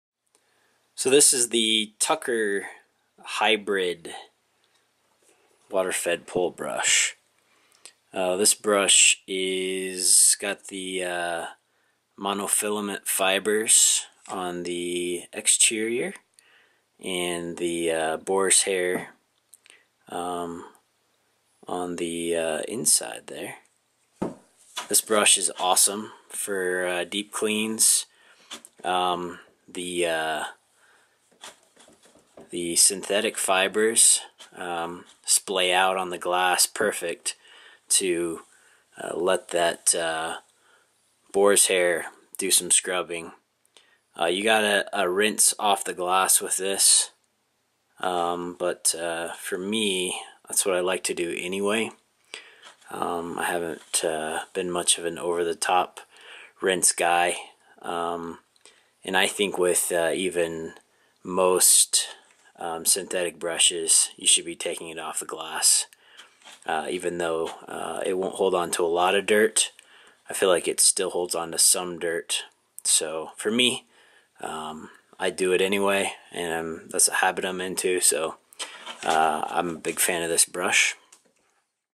Tucker Hybrid Brush
Dual trim hybrid brush with nylon and boars hair is excellent for both first cleans and maintenance cleans.